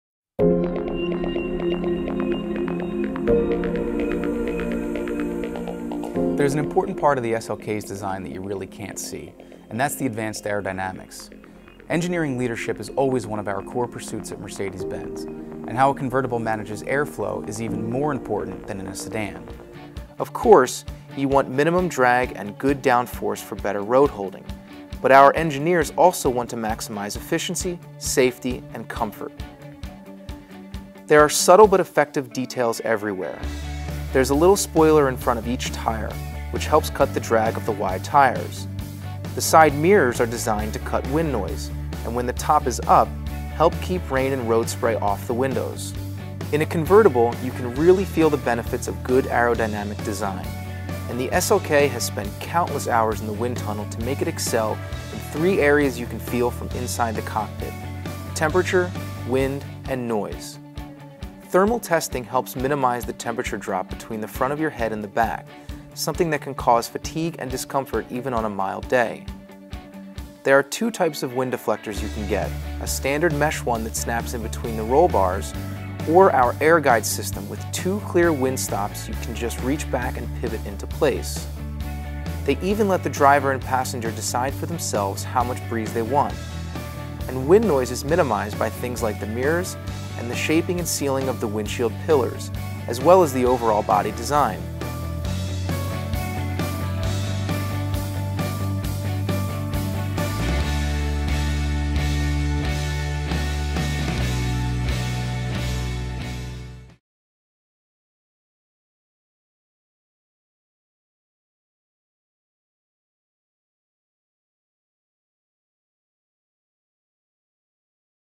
SLK Roadster Exterior Features -- Mercedes-Benz Hardtop Convertible
The SLK's gorgeous physique is influenced by a rich racing heritage, but it also takes cues from countless hours in the wind tunnel. The unique body shape is combined with aerodynamic lower work to minimize drag and maximize efficiency. That means the SLK helps to cut down on wind and noise for the driver, resulting in a rewarding drive at any and every speed.